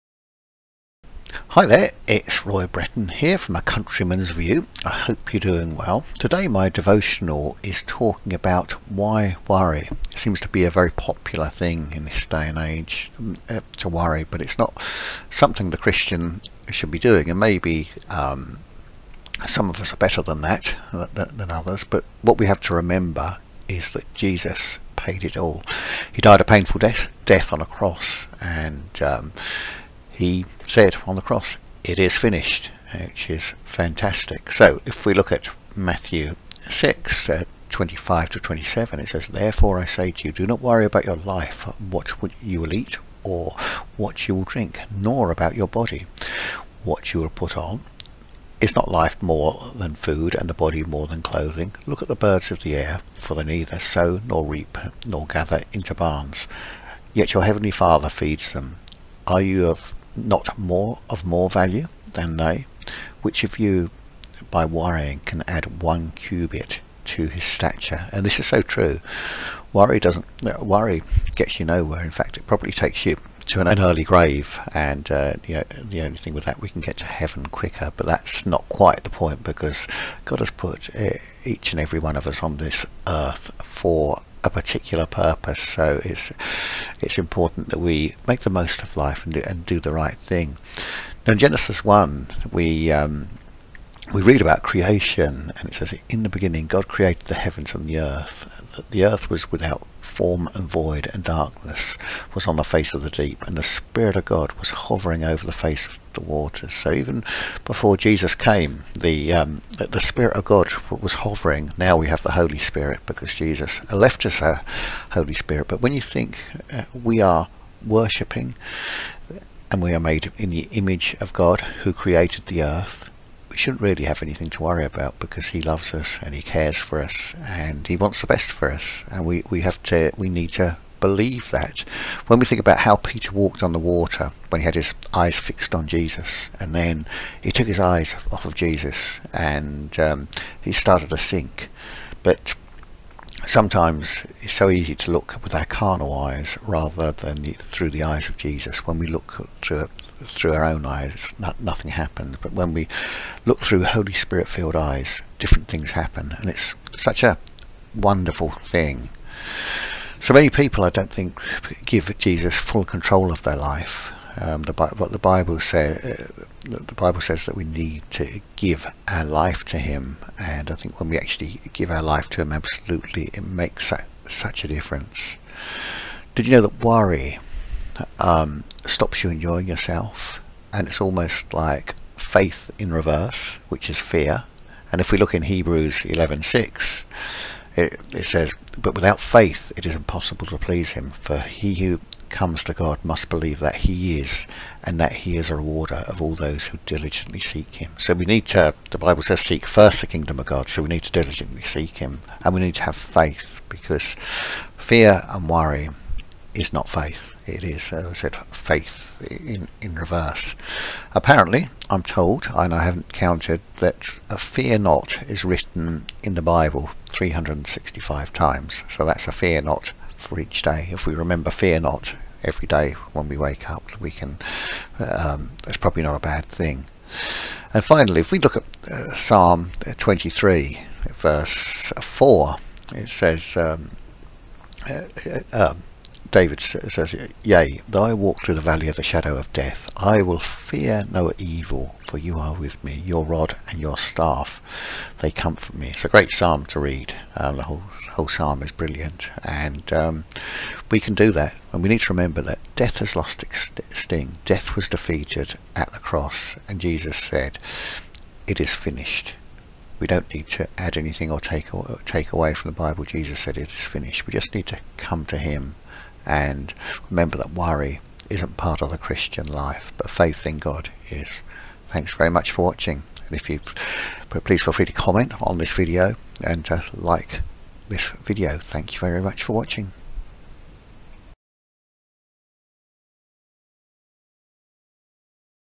Christian Devotional – Why Worry?
In today's devotional, Roy Bretton from A Countryman's View explores the topic of worry and why it's something Christians shouldn't be burdened with. Drawing from Matthew 6:25-27 and Psalm 23, Roy reminds us that Jesus has already paid the price and that worrying doesn't add any value to our lives. Instead, we should place our trust in God, who cares for us and wants the best for us.

Roy also discusses the importance of giving full control of our lives to Jesus, the dangers of letting fear overshadow faith, and the power of remembering that Jesus' victory on the cross means that death has lost its sting.
Join Roy as he encourages us to live by faith, not by worry, and to remember that "fear not" is written 365 times in the Bible—one for each day of the year.